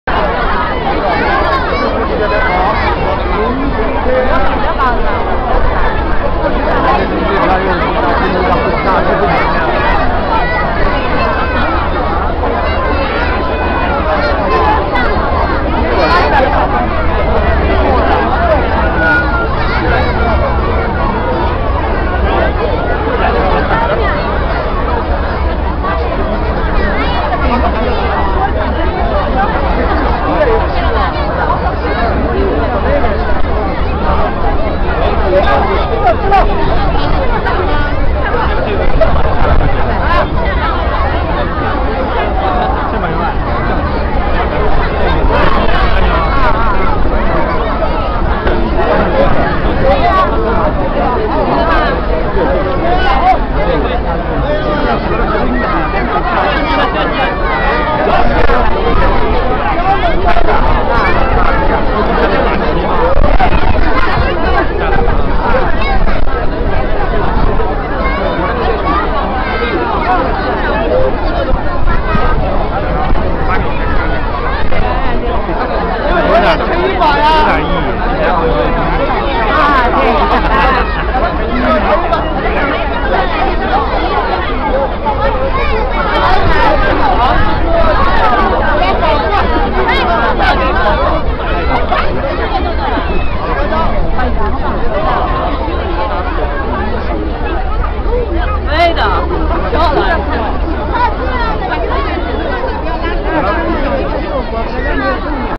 Flag Ceremony in Beijing Part 1
Here is the first part of the soldiers taking down the flag in Tiananmen Square in Beijing... I couldn't see ANYTHING standing there, so these videos are the only way I could see what was going on. Haha